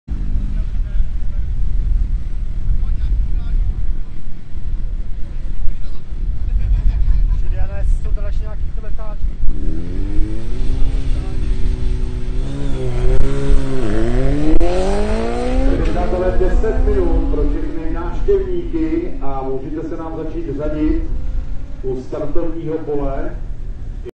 Suzuki B-king solo Drag Race 1/4M (402m), Kámen u Pacova, 8.7.2017, automotosprint.cz
Suzuki B-king solo Drag Race 1/4M (402m)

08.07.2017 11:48

#60 Suzuki B-king
Kategorie: m2
čas 1/4m: 10.452s
rychlost 1/4m: 225.19km/h

Kámen u Pacova, 8.7.2017